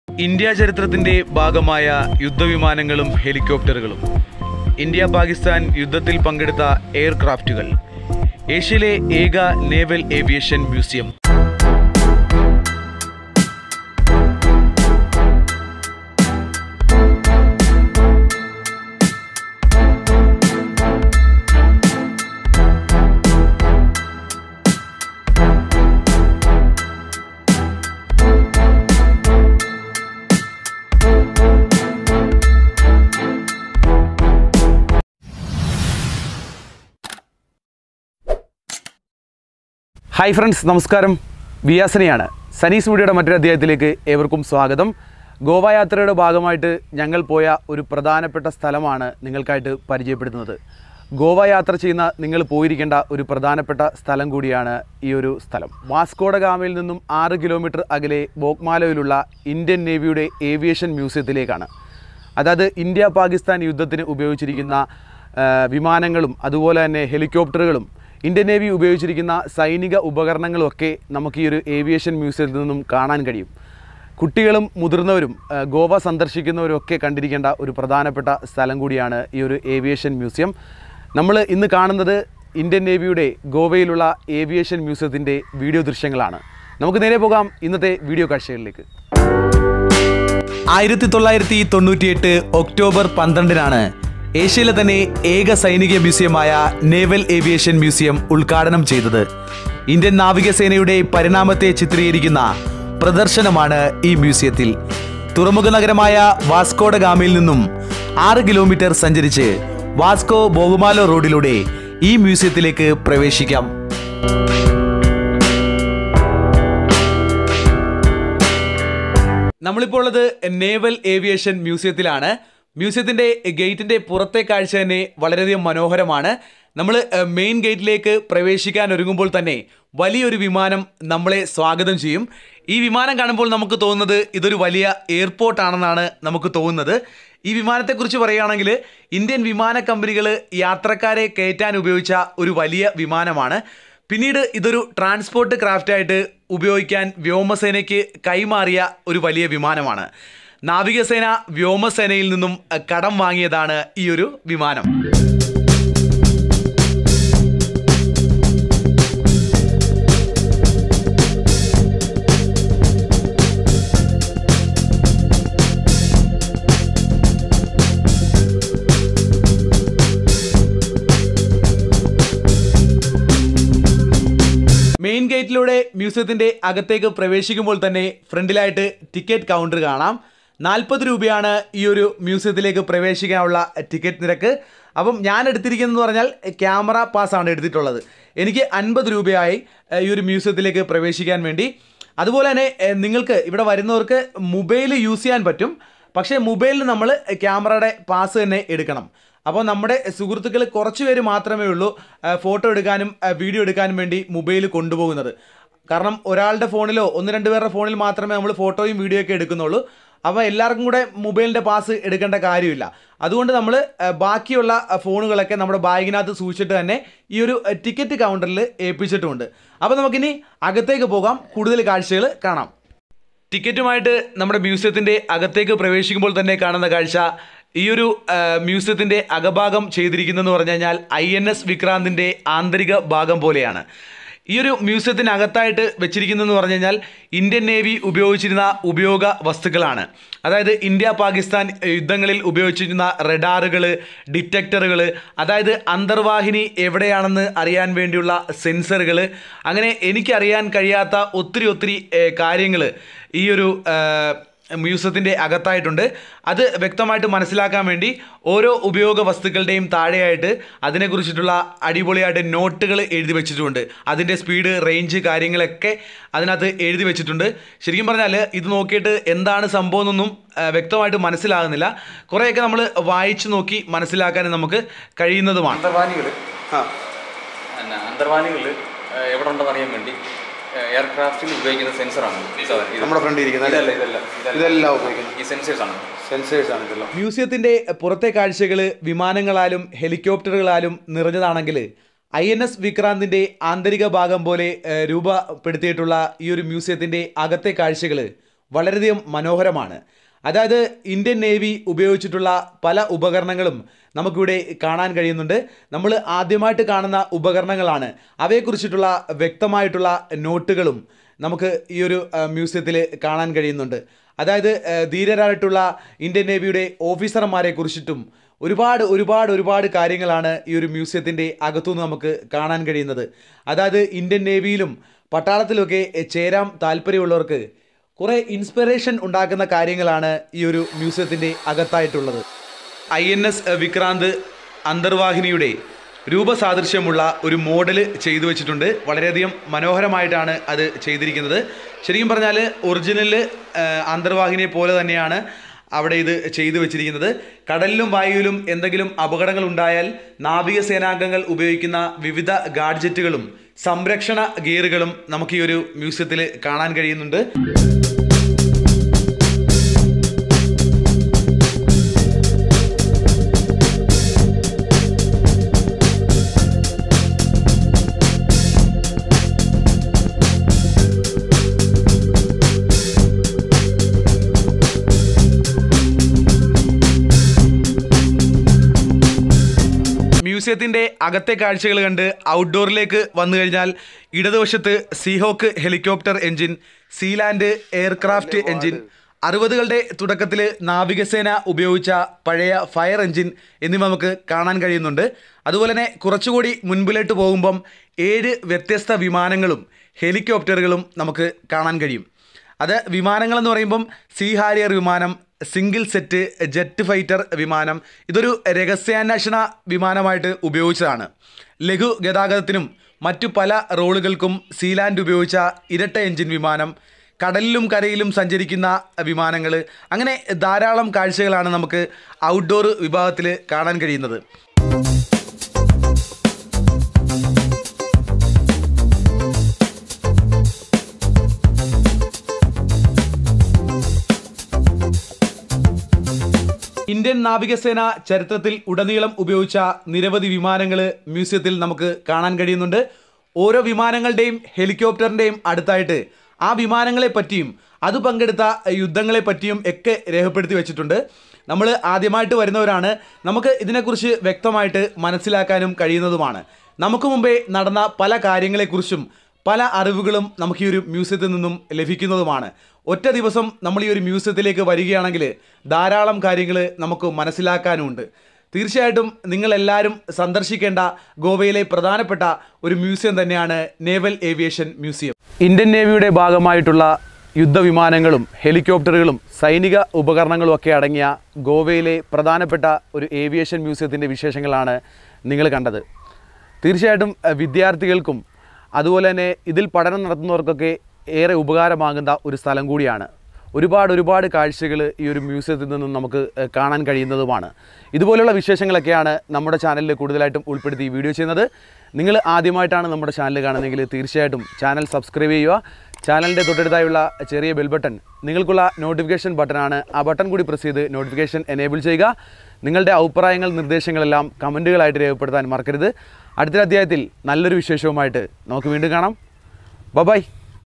Goa Trip | Goa Naval Aviation Museum | ഗോവ യാത്രയിൽ പോയ നേവൽ ഏവിയേഷൻ മ്യൂസിയം | Sani's Media
ഗോവ യാത്രയിൽ പോയിരിക്കേണ്ട പ്രധാന സ്ഥലങ്ങളിലൊന്നാണ് നേവിയുടെ ഏഷ്യയിലെ ഒരേയൊരു സൈനിക മ്യൂസിയമായ നേവൽ ഏവിയേഷൻ മ്യൂസിയം വാസ്കോഡഗാമയിൽ നിന്നും 6 കിലോ സഞ്ചരിച്ച് വാസ്കോ ബോഗ്മാലോ റോഡിനടുത്താണ് ഈ മ്യൂസിയം സ്ഥിതി ചെയ്യുന്നത് indian നാവികസേനയുടെ പരിണാമത്തെ ചിത്രീകരിക്കുന്ന പ്രദർശനമാണ് ഗോവയിലെ ഈ മ്യൂസിയത്തിലുള്ളത്

Location Map
Indian Naval Aviation Museum